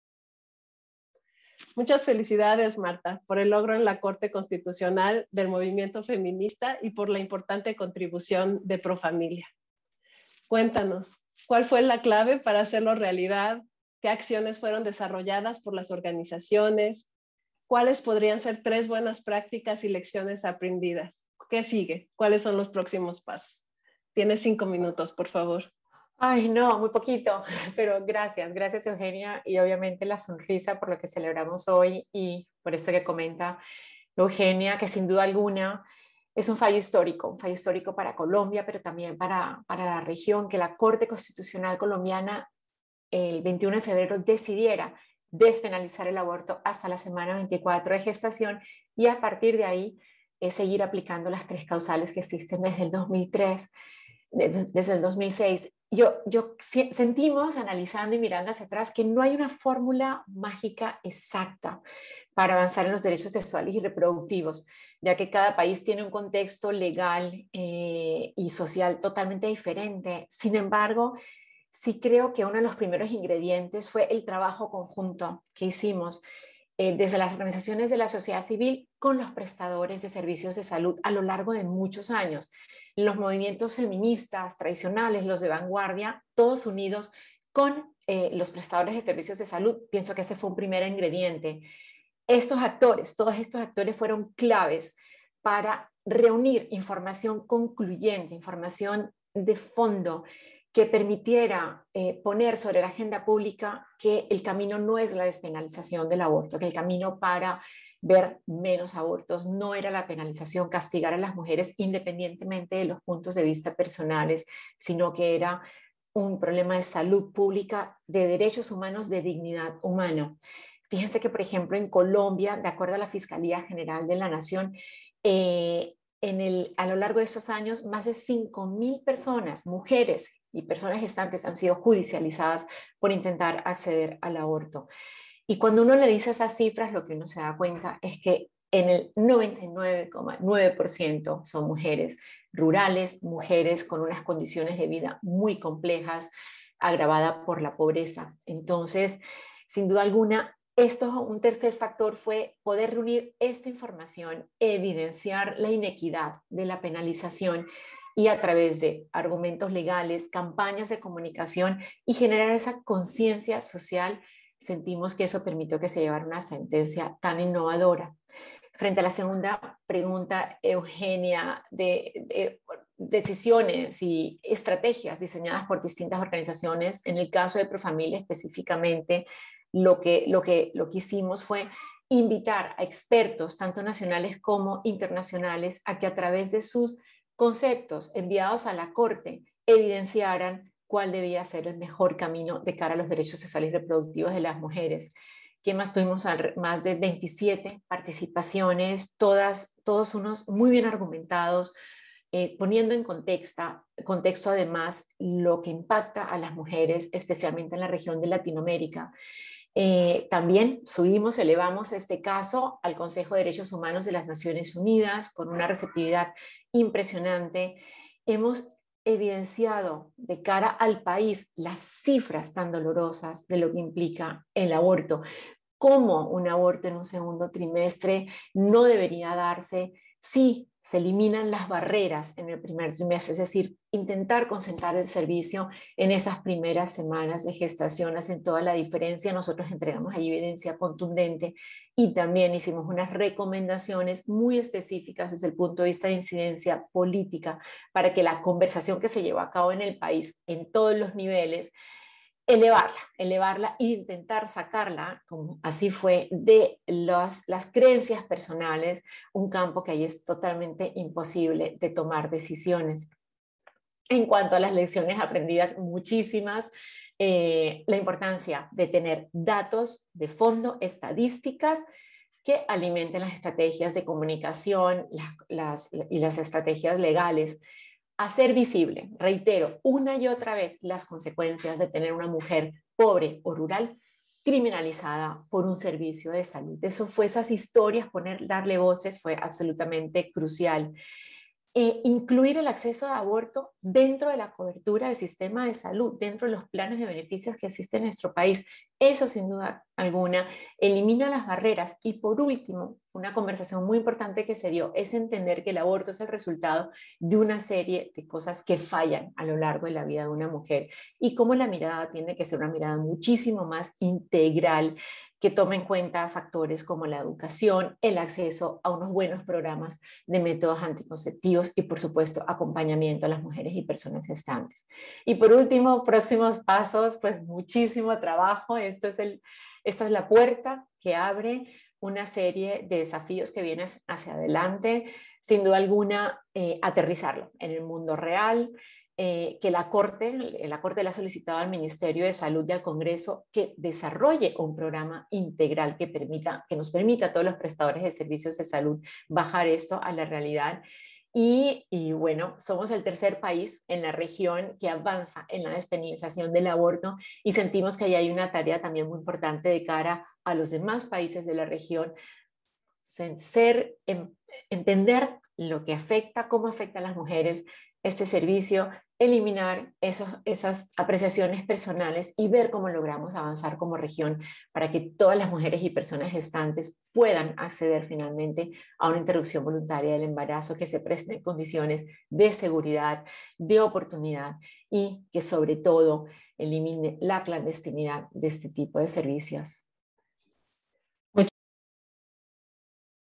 Marta Royo Directora Ejecutiva Profamilia, Colombia - Día Internacional de la Mujer 2022
Celebrando la Autonomía Corporal y Empoderamiento de las Mujeres y Niñas Organizado por la Oficina Regional de las Américas y la Oficina, en conmemoración del Día Internacional de la Mujer 2022